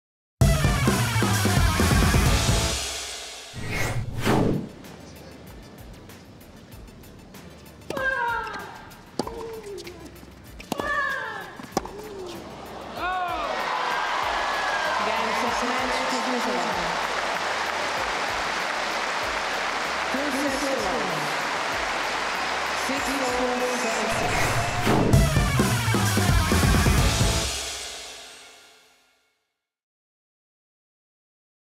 Match point: Kuznetsova d Henin
Svetlana Kuznetsova completes a two-set win over Justine Henin on day five of Australian Open 2011.